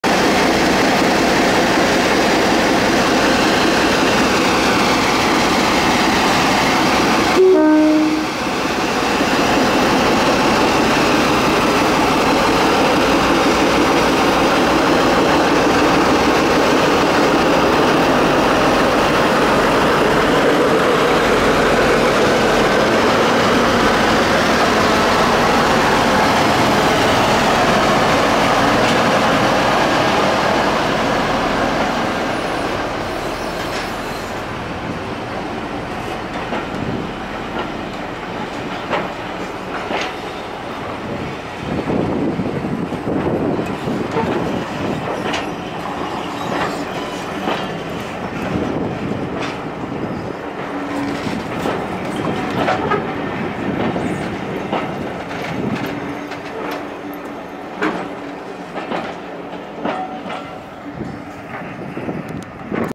D8568 Tenterden 12 4 24
The Sole surviving Clayton class 17 D8568 takes the stock out towards St Michaels headshunt.